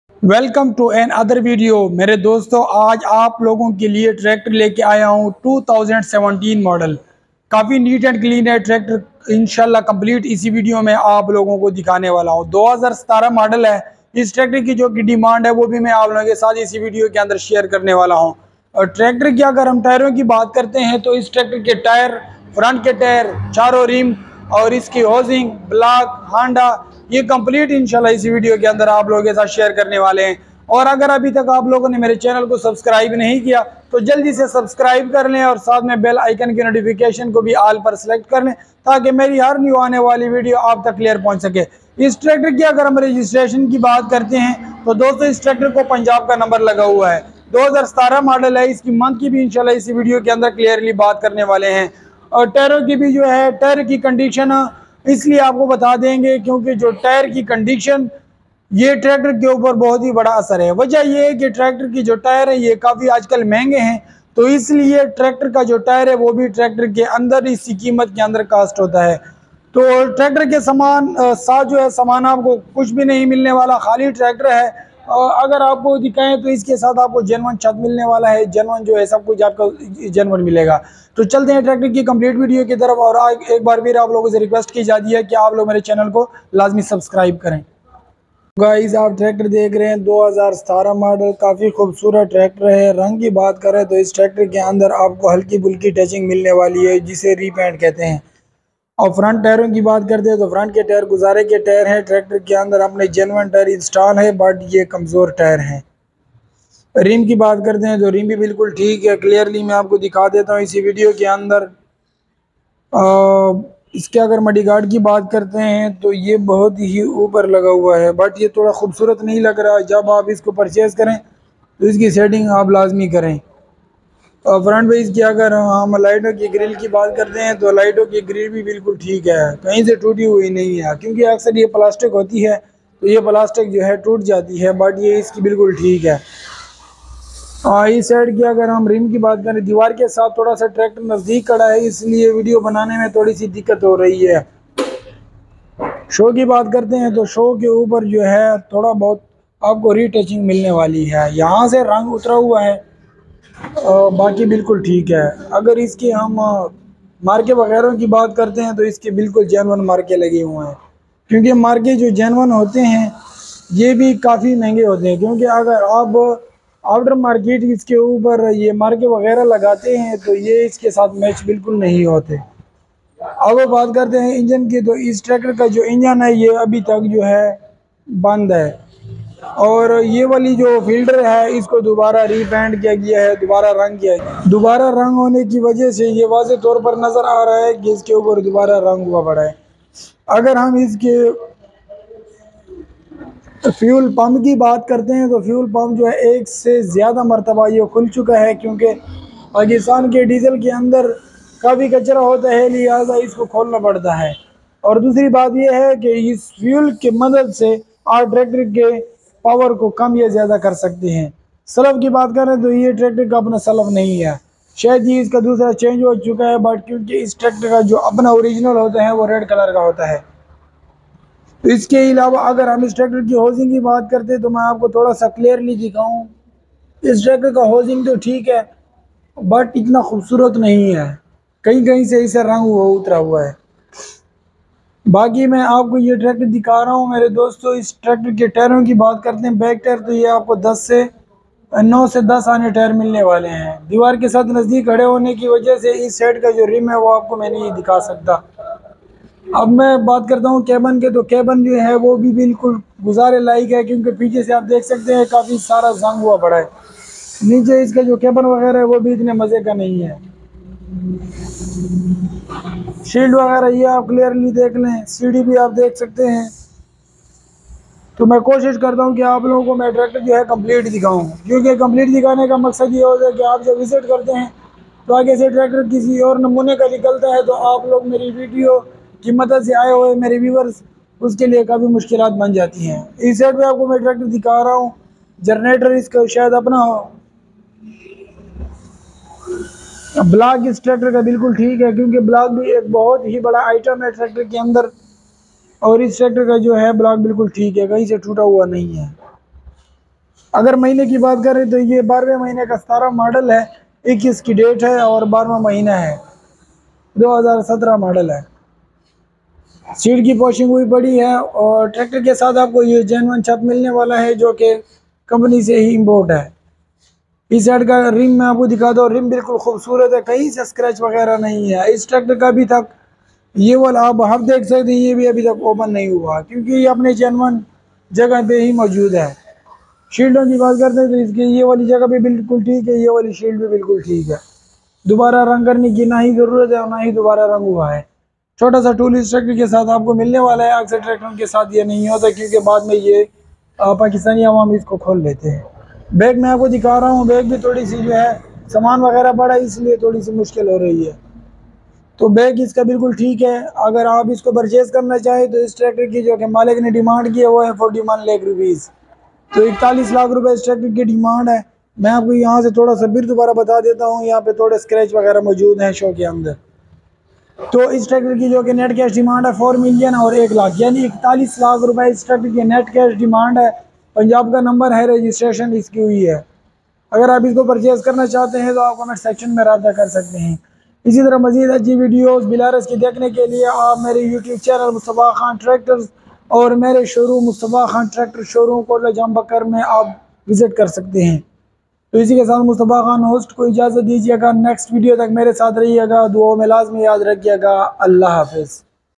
Belarus tractor 510 for sale in pakistan |russi tractor |complete review today price روسی ٹریکٹر
Belarus tractor 510 model 2009 for sale in pakistan russi tractor purchase kary
Russian tractor with complete requird material